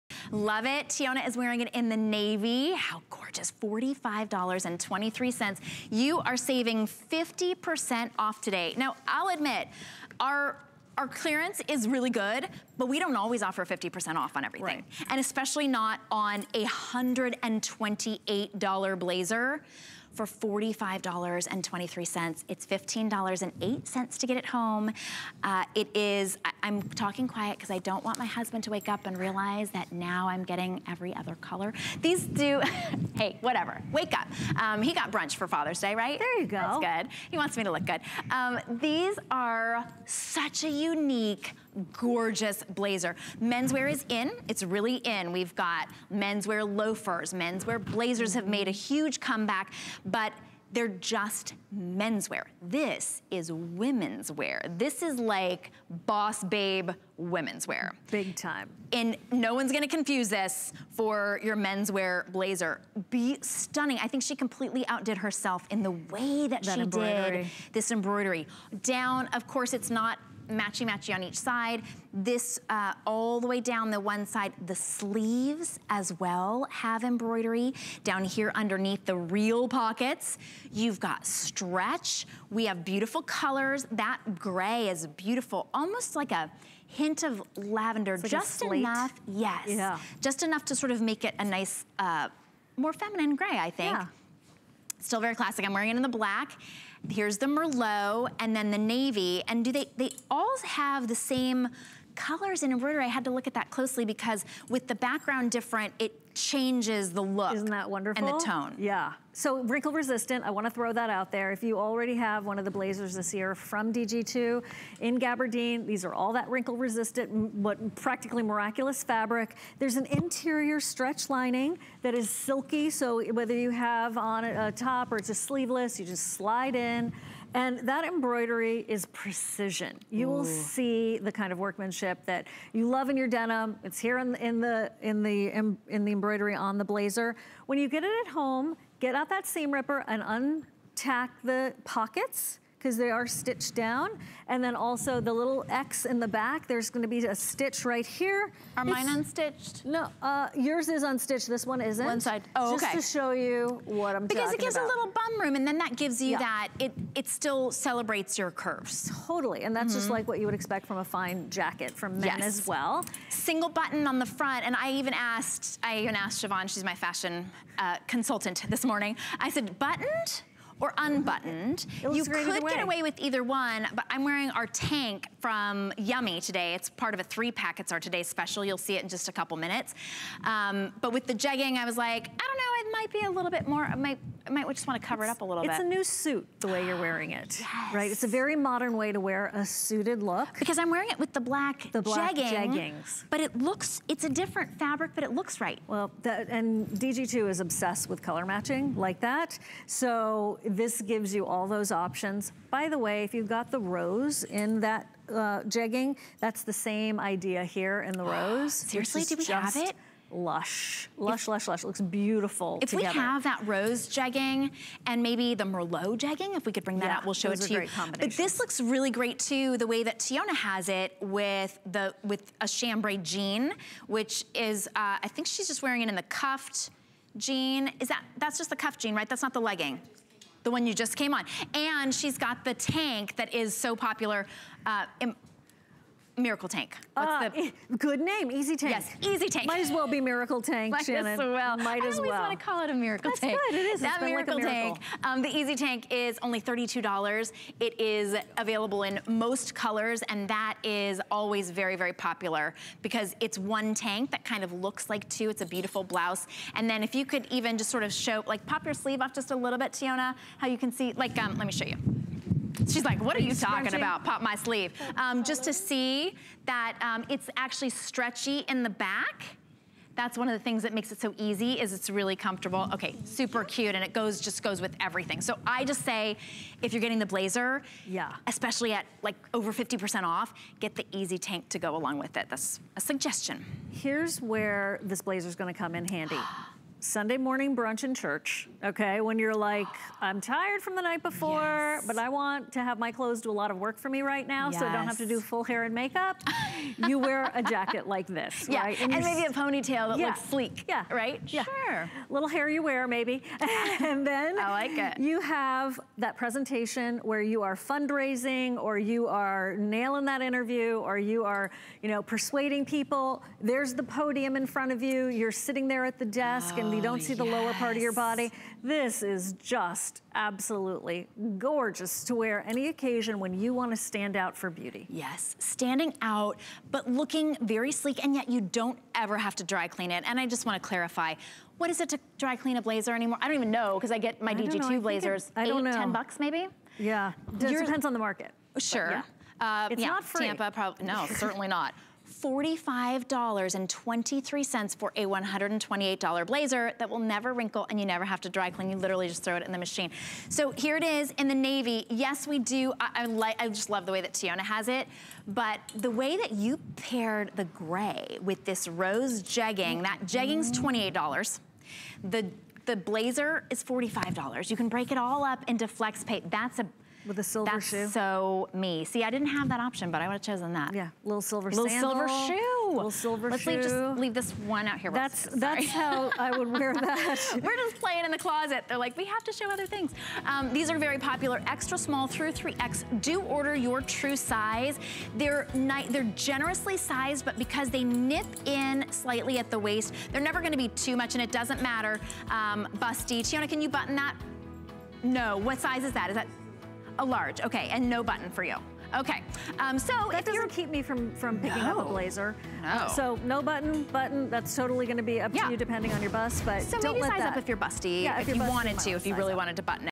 DG2 by Diane Gilman WrinkleResistant Embroidered Gabardi...
DG2 by Diane Gilman WrinkleResistant Embroidered Gabardine Blazer
Floral embroidery jazzes up the tailored look of this gabardine blazer, adding a trendright touch to set it apart. Perfect for officetodinner dressing and for and  special events.